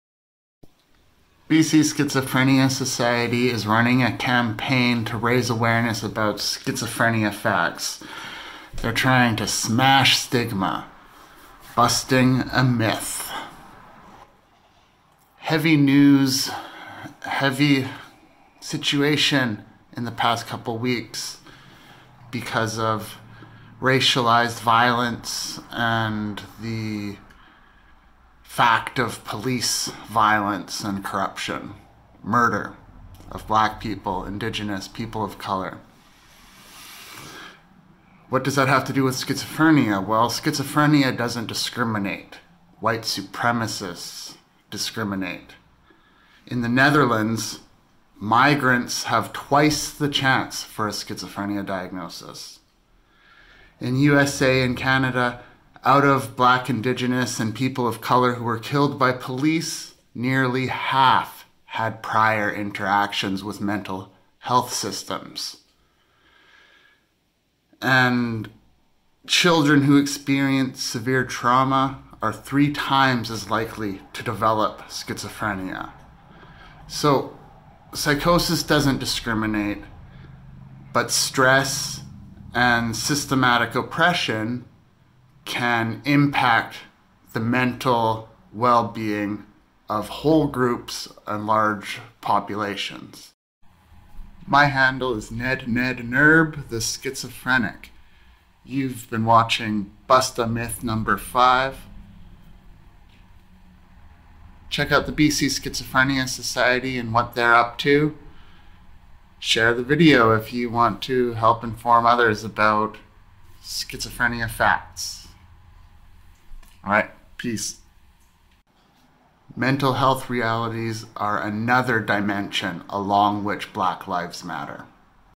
Ignored And Magnified: Unveiling The Unequal Impact Of Schizophrenia & Rooting Out Discrimination
Myths In Mental Health Stigma.
Myth 5 - A fact check on discrimination. In actuality, schizophrenia does not affect every population exactly equally. Every group on Earth displays SZ symptoms, but not all evenly.

Namely, there is a larger incidence of SZ in populations such as Black, Indigenous, and People of Color who experience discrimination and oppression. Early and ongoing trauma and abuse contribute to mental illness and symptoms.

It is time to protest police brutality, defund the police violence, and end systemic racism. As a white person, I do what is in my power to support those white people learning around me and uphold marginalized, oppressed voices of BIPOC.

BC Schizophrenia Society is running a campaign to raise awareness about myths and facts regarding schizophrenia.

This has been a production of

Mostly, in my videos I discuss schizophrenia, mental health topics, and philosophy.

Music I made or helped make:
nerBeater